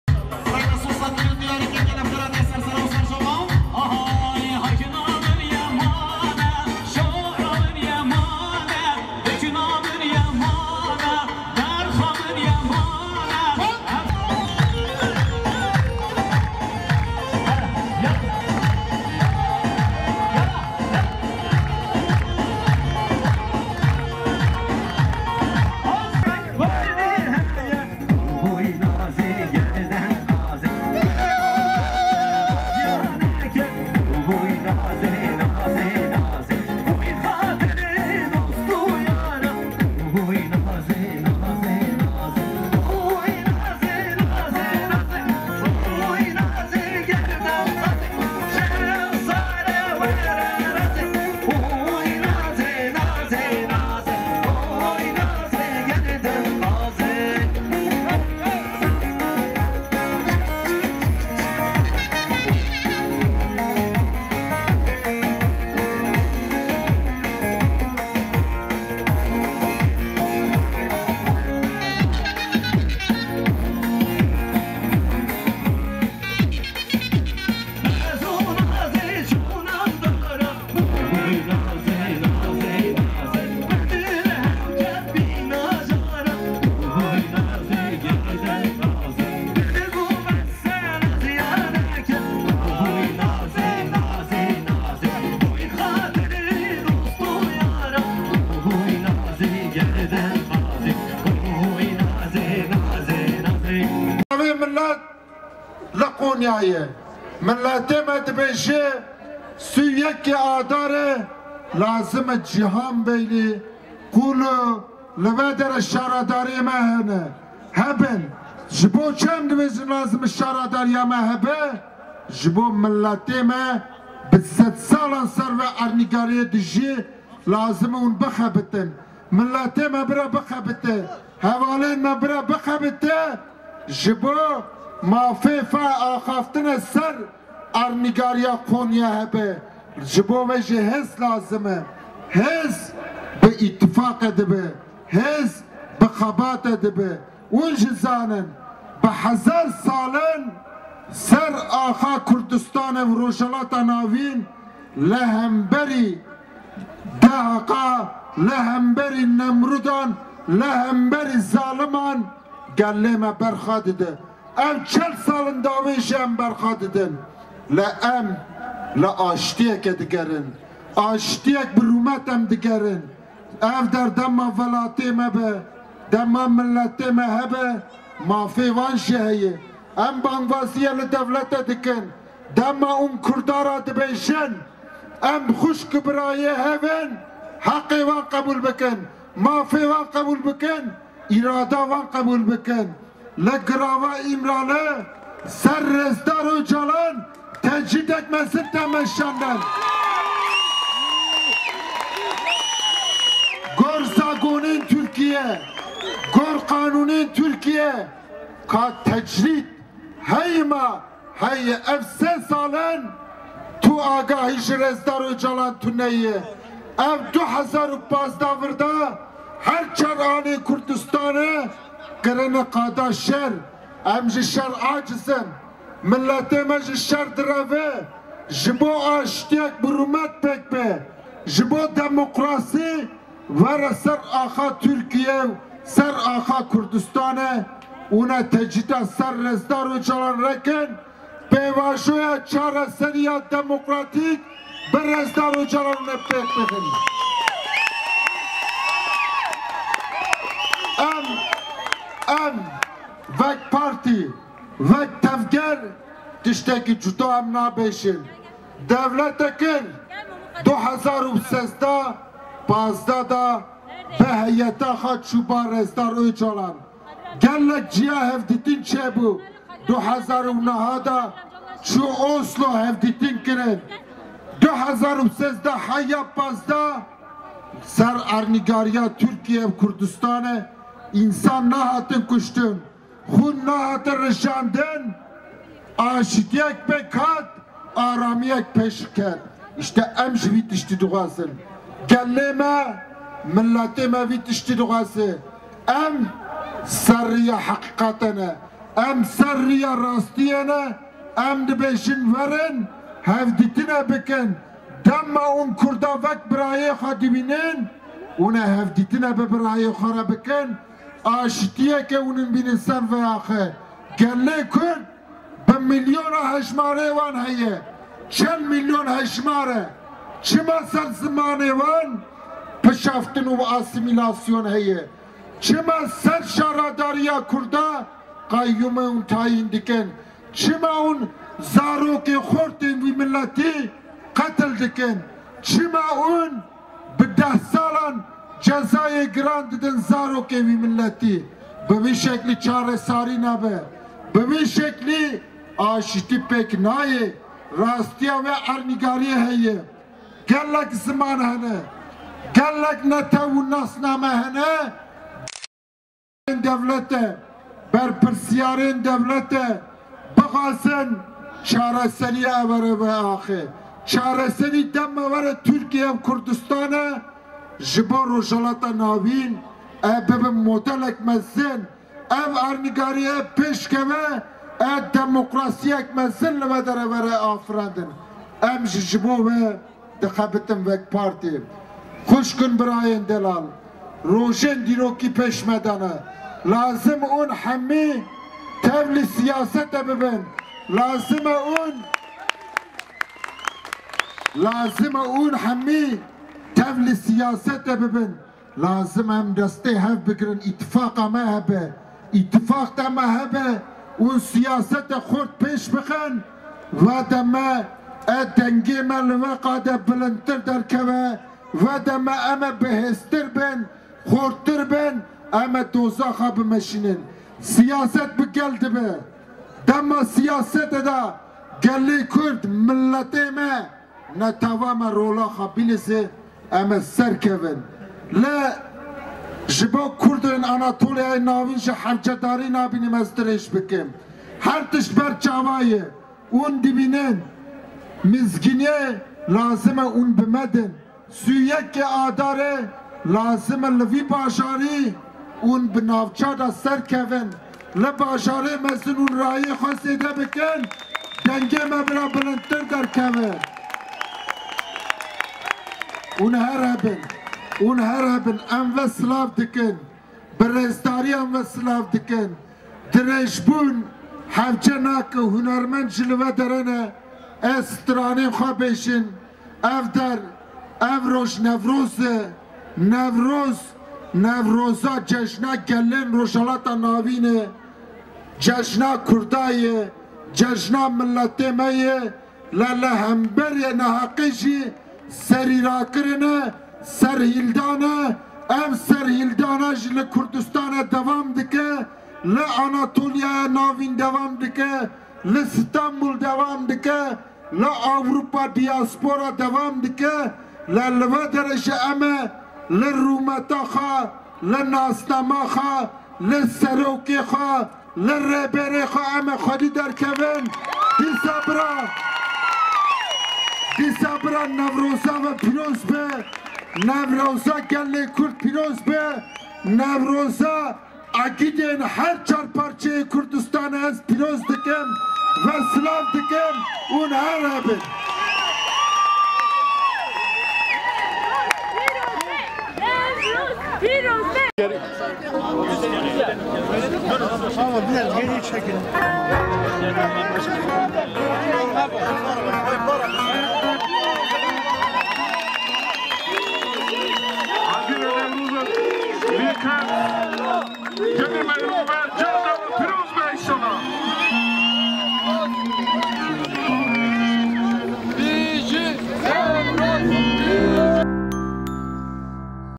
KONYA'DA NEWROZ KUTLAMASI
Konya’da Newroz, Valiliğin programın başlamasına 3 saat kala verdiği izinle Uğurlu Salı Pazarı’nda kutlandı. Alanı dolduran yüzlerce kişi arasında kadınlar, gençler ve çocuklar yoğunluğu dikkat çekti. Kadınların kutlamaya yöresel kıyafetlerini giyerek geldikleri görülürken alana kutlamaların bu yıl ki şiarı olan “Rabe, dema azadî û serkeftinê ye, Şimdi özgürlük zamanı” pankartı asıldı.